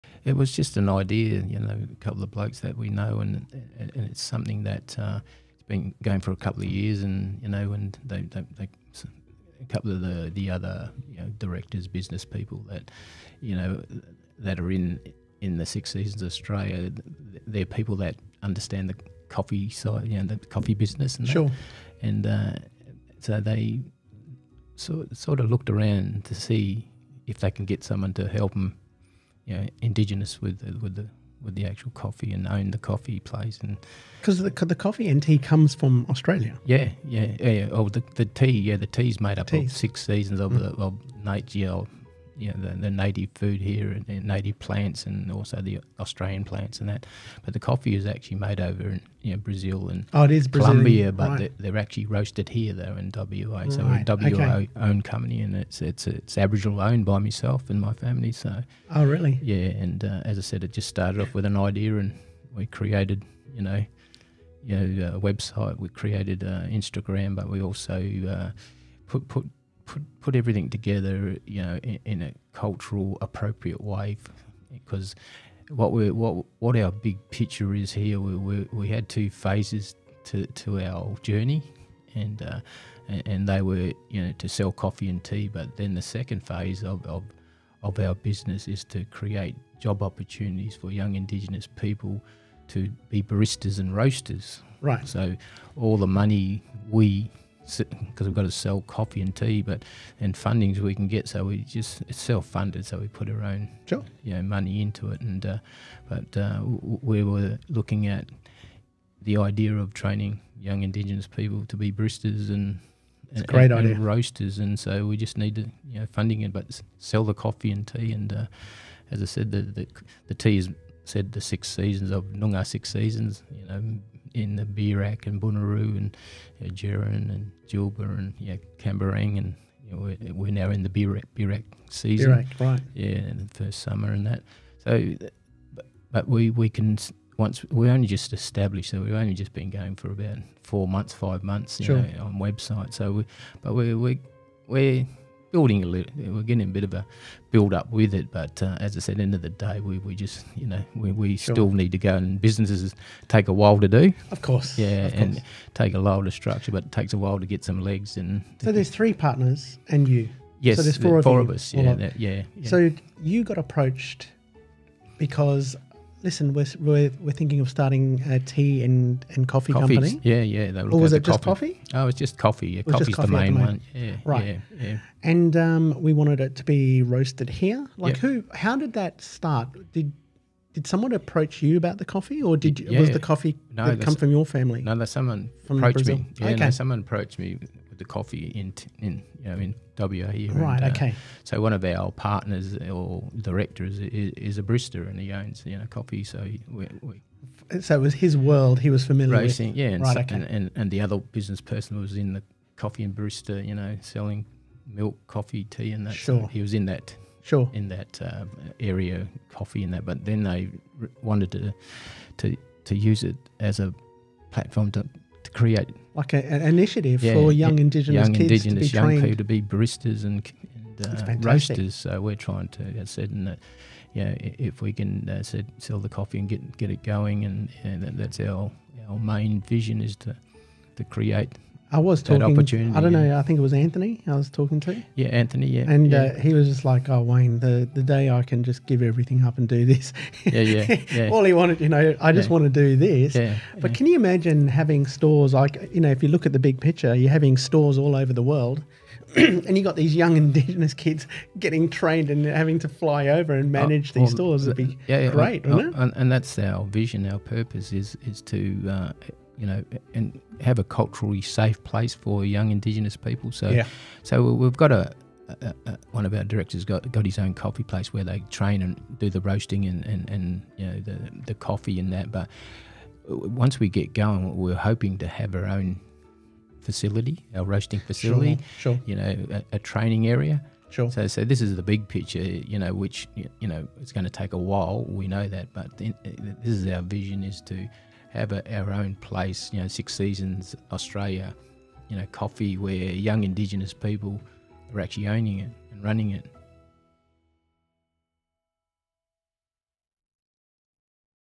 Six Seasons Australia : A coffee and tea company | Phil Narkle
Episode 22 - Phil Narkle
The Wayne Nicholson Show

Phillip Michael Narkle is a former Australian rules football player of Indigenous Australian descent who played for St Kilda and West Coast in the Australian Football League and Swan Districts Football Club in the West Australian Football League during the mid-late 1970s and early 1990s. Phil now has teamed up with 3 partners in a company called Six Seasons Australia; a coffee and tea company with the incentive of employing and training indigenous Australians to becoming professional barristers.

Podcast available on:
iTunes, Spotify & most streaming platforms

My Social Feeds:
Facebook - The Wayne Nicholson Show